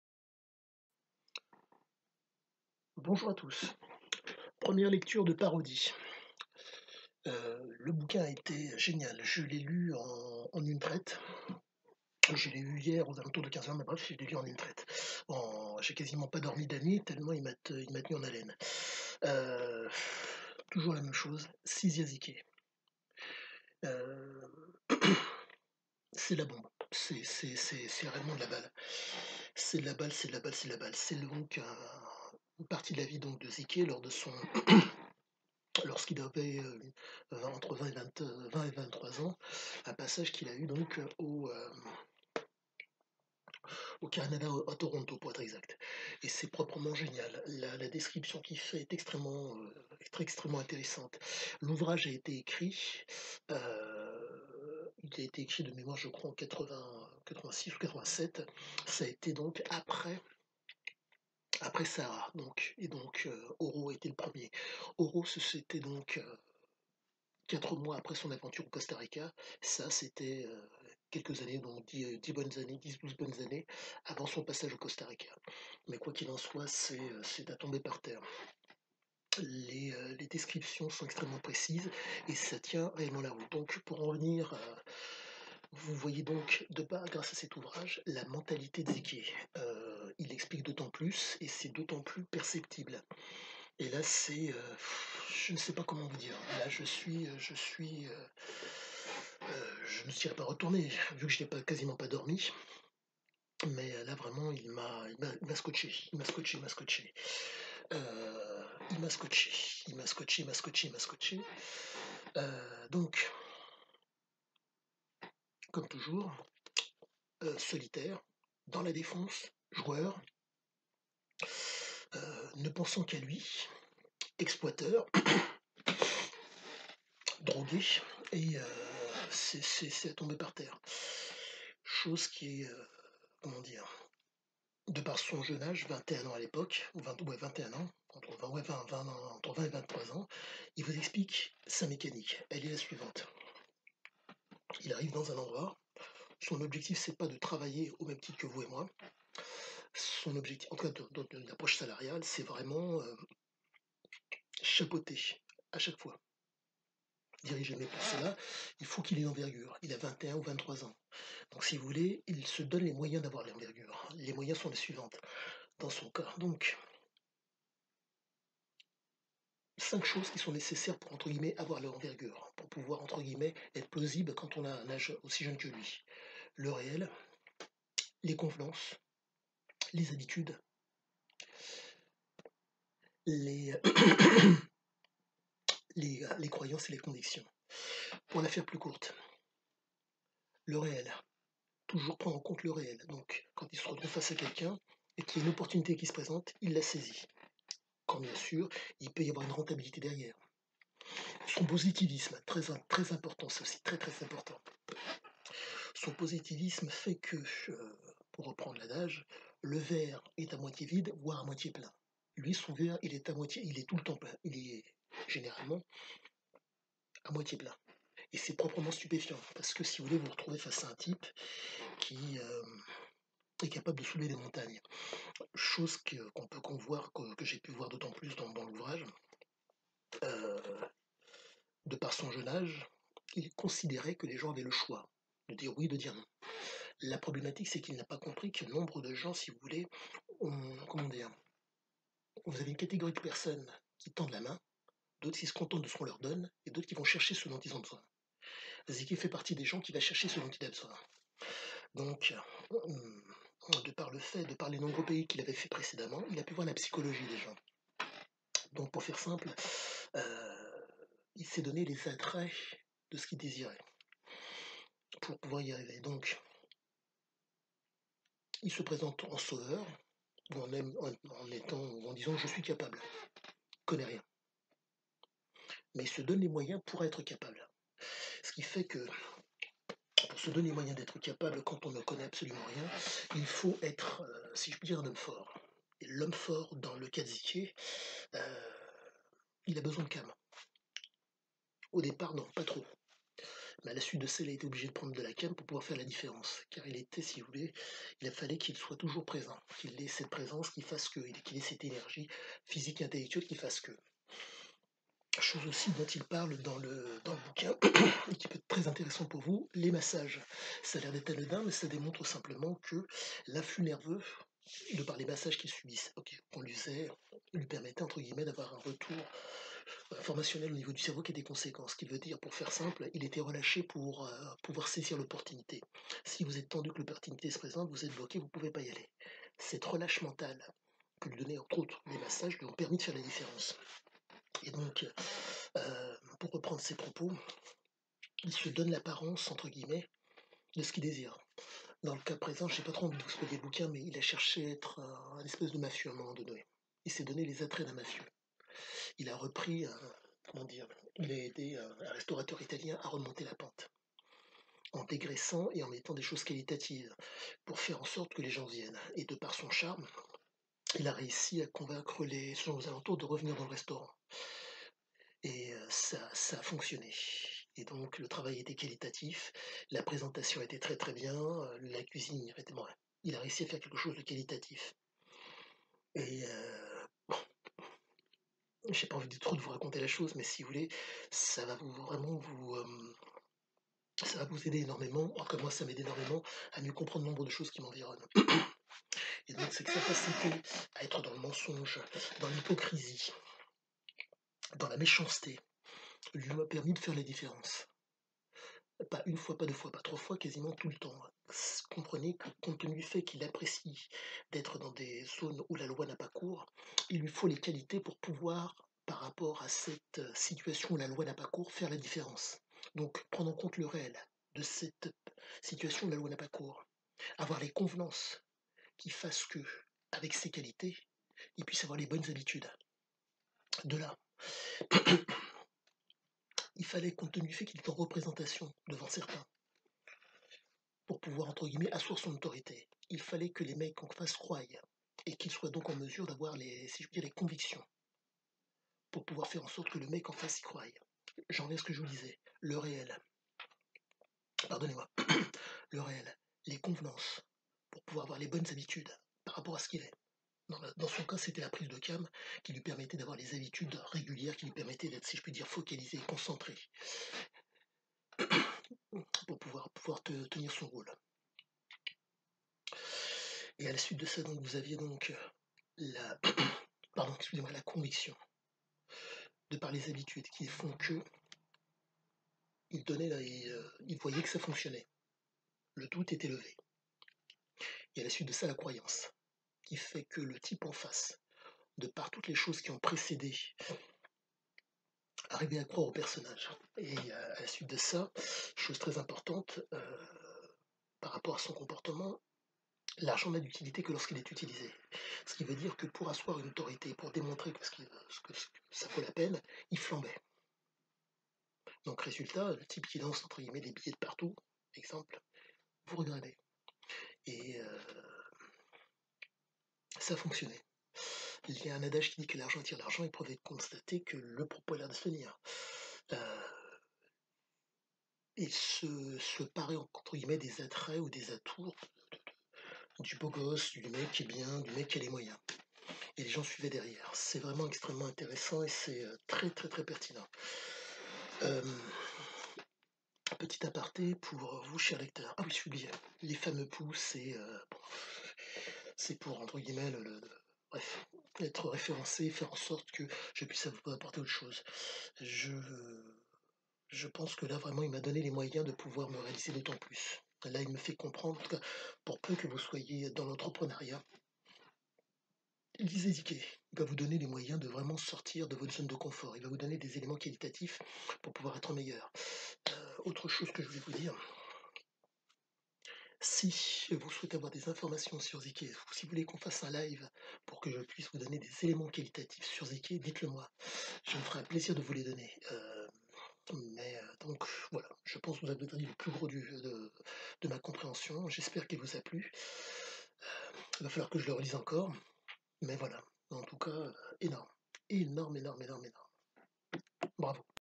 Merci ZYKE 1er lecture de parodie
De 19H à 4H30 je l'ai lu, il explique la génèse de ce qu'il était, que vous soyez femme homme, adolescent lisez le, nombre de chose vous étant arrivé ou que vous vivez, sans pour autant les comprendre ou vous les expliquez.Vous paraîtrons plus limpide.
Le lien pour acheter un livre audio de Zyke